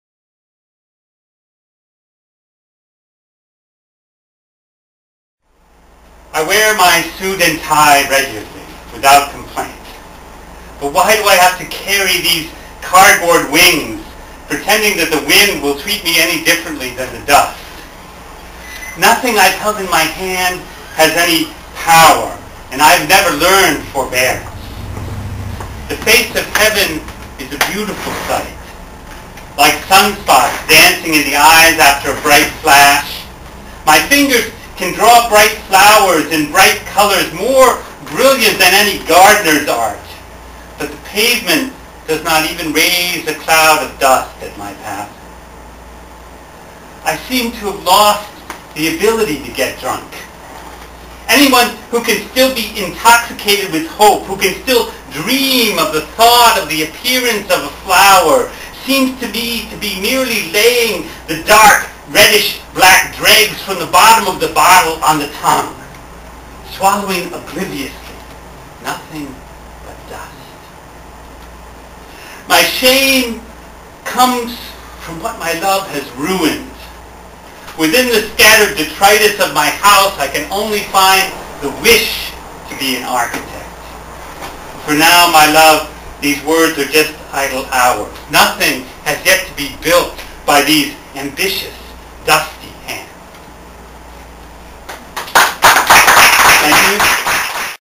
Stone Soup Poetry Presents Douglas Bishop
From Stone Soup Poetry's Open Mike on September 7, 2009.